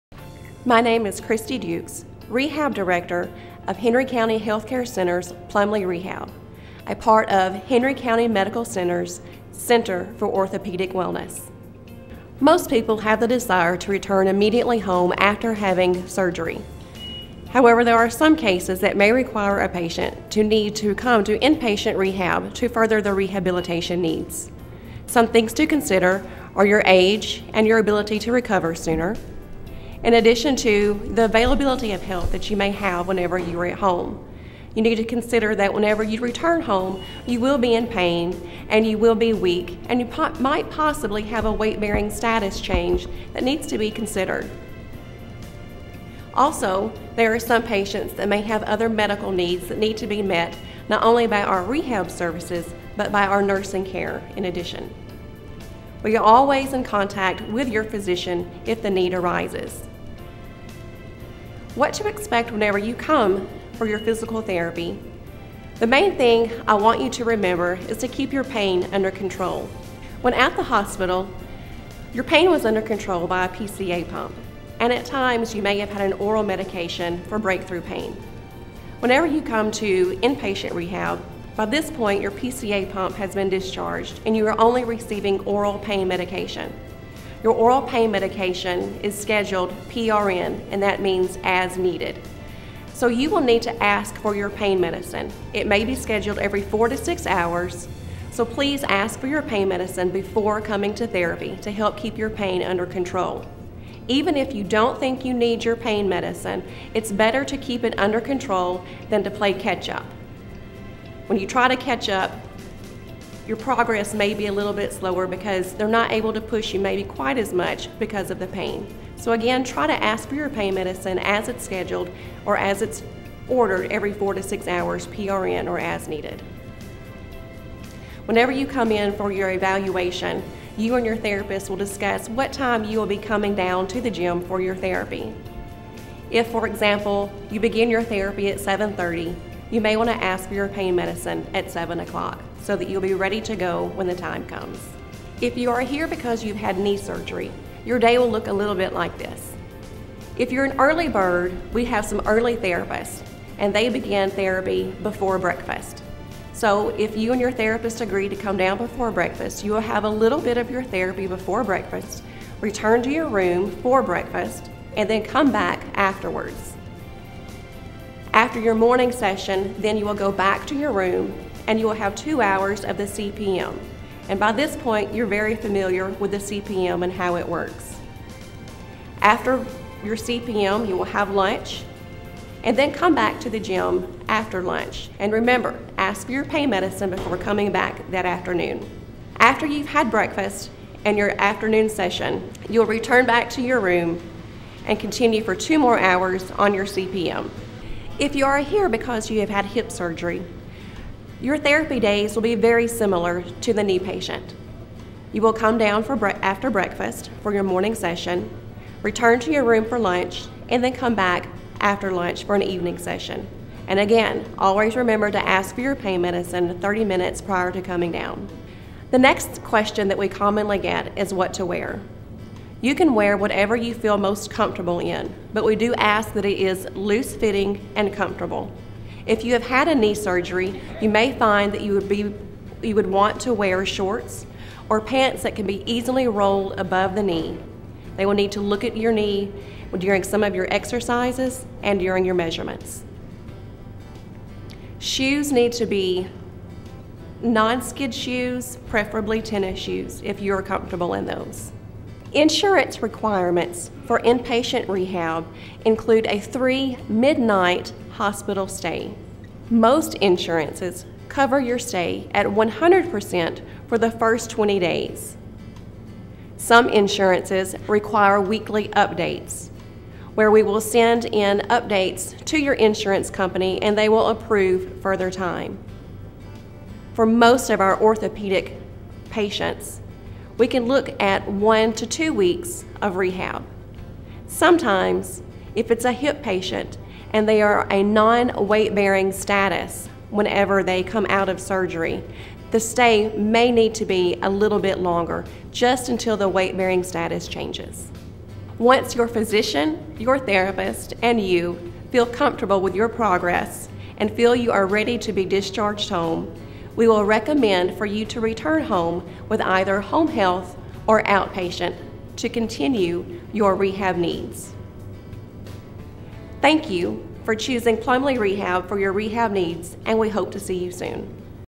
What to Expect at Plumley Rehabilitation after Joint Surgery
Christy Dukes, Director of Plumley Rehabilitation, discusses the care a patient will receive at Plumley including the daily routine, exercises, and much more.